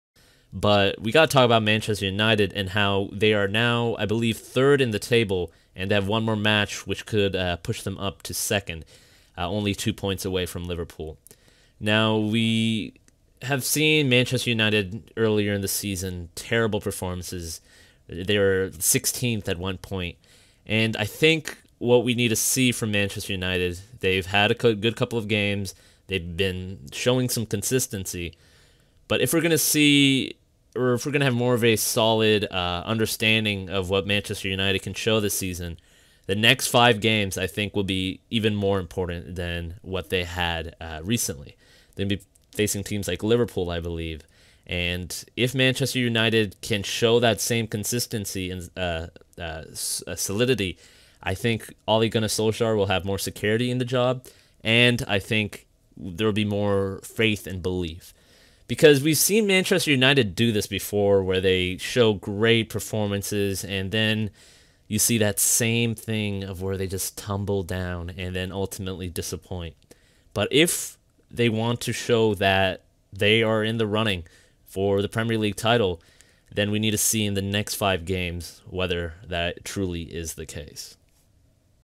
Man United need to prove title challenge in next 5 games
Manchester United have rebounded in their last five and are now at third in the Premier League. However, if they are trying to mean business Man United will need to show that in the next 5 games. With Man United have had a history of flopping after a strong showing, these could prove whether they are being consistent.